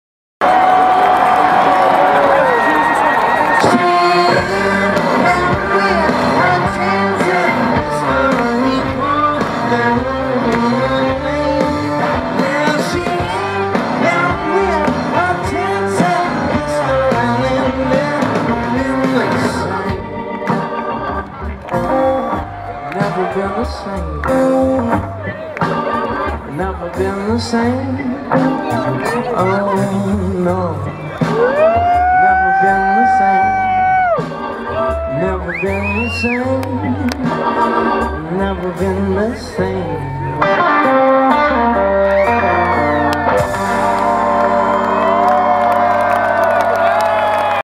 The Black Keys Live at Lollapalooza 2012
The Black Keys perform live at Lollapalooza 2012 on Friday, August 3, 2012.